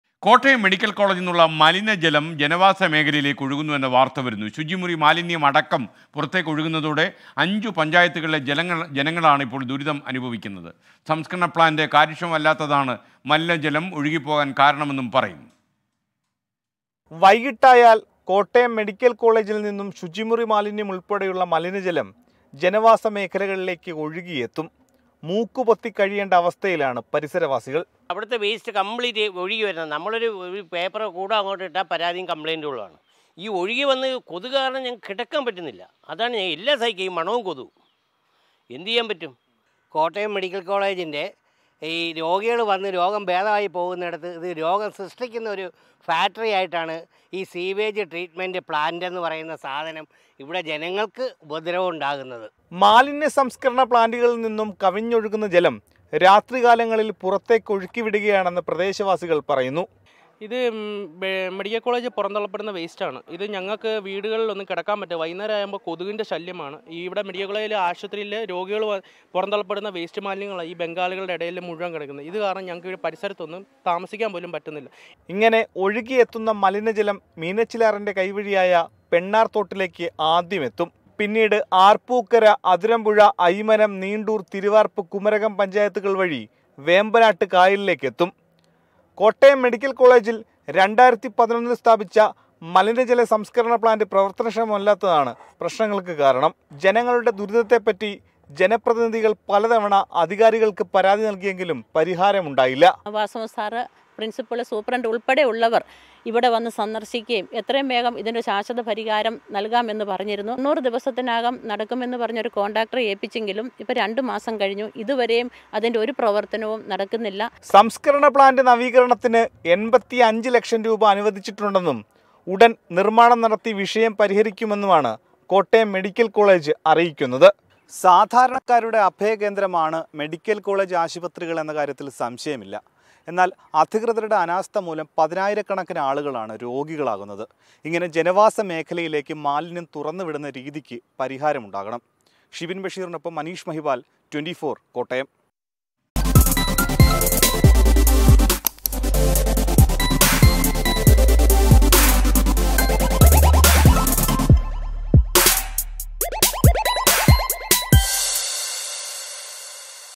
മെഡിക്കൽ കോളേജിൽ നിന്നുള്ള മലിനജലം ജനവാസ മേഖലയിലേക്ക്
മെഡിക്കൽ കോളേജിൽ നിന്നുള്ള മലിനജലം ജനവാസ മേഖലയിലേക്ക്; ശുചിമുറി മാലിന്യമടക്കം പുറത്തേക്ക് ഒഴുകുന്നതോടെ അഞ്ച് പഞ്ചായത്തുകളിലെ ജനങ്ങളാണ് ദുരിതമനുഭവിക്കുന്നത്
Sewage from medical college to residential area; The people of five panchayats are suffering as the toilet waste is flowing out